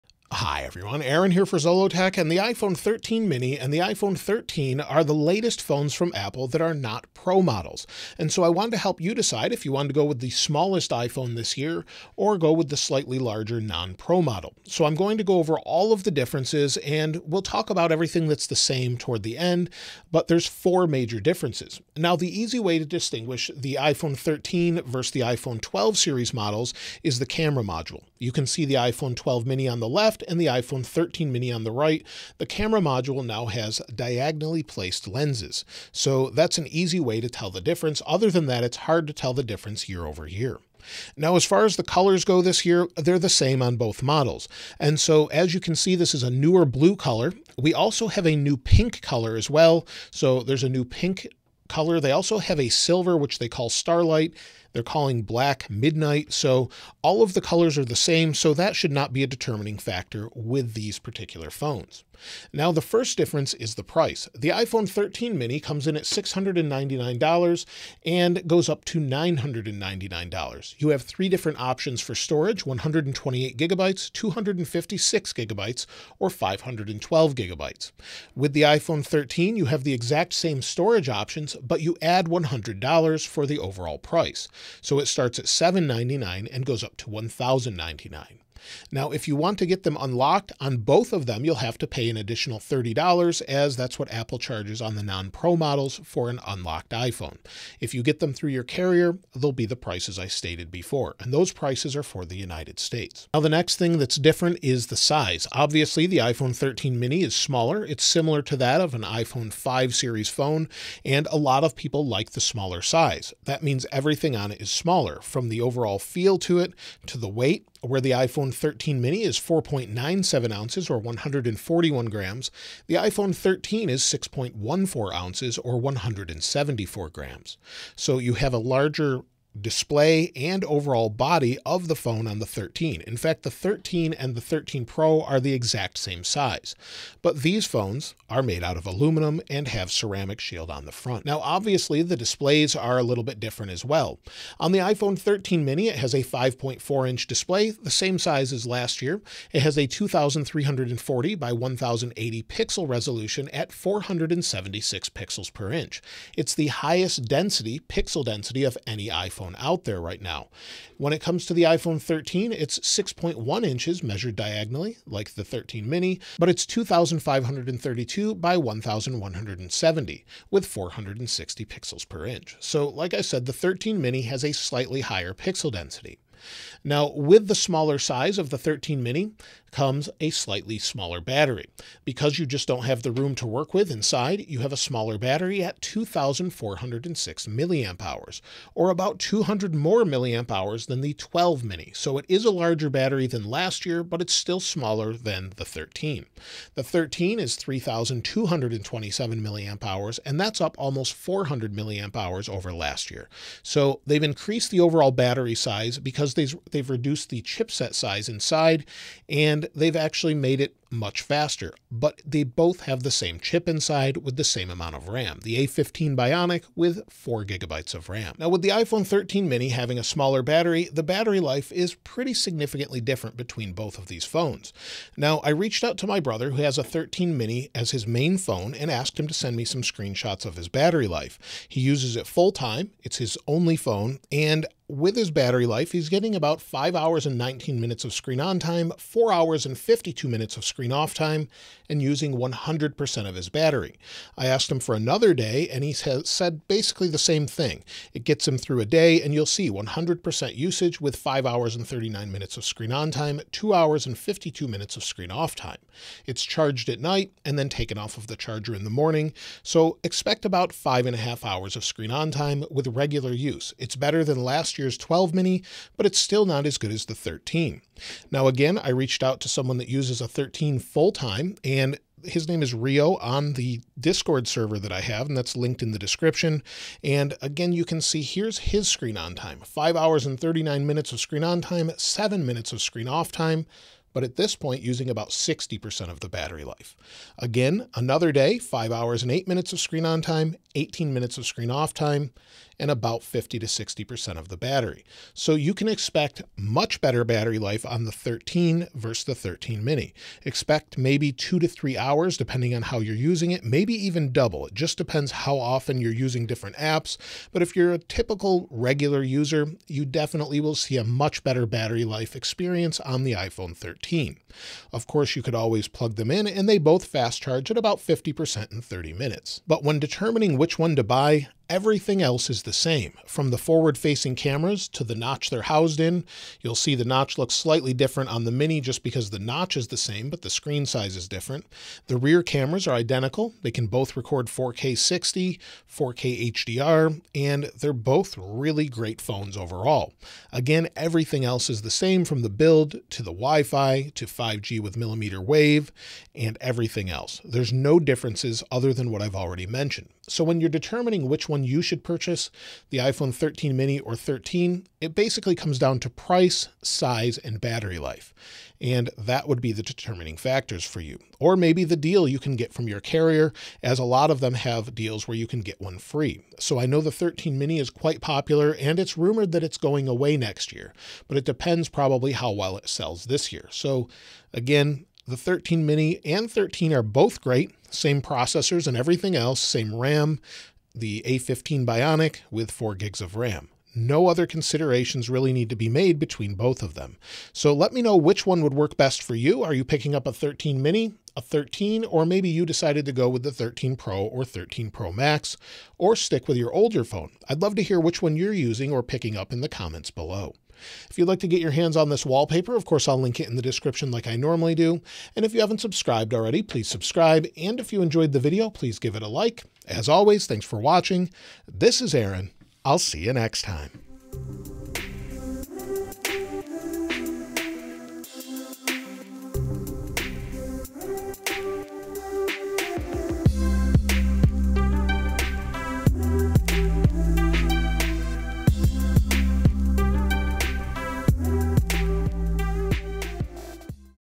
iPhone 13 vs iPhone 13 mini - Which is Best for You?
iPhone 13 mini and iPhone 13 have some differences, but also have a lot more similarities than anything else. I go over what is different and what is the same between iPhone 13 mini and iPhone 13. I help you decide which iPhone 13 model is the right choice for you. We compare the price, the display, talk about the camera and much more.

iPhone 13 mini on Amazon
iPhone 13 on Amazon

***Time Codes***
00:00 - Introduction
00:40 - Price
01:42 - Size and Weight
02:56 - Battery size
03:38 - Battery life
05:19 - Similar Features
05:50 - Which should you choose?
06:54 - Outro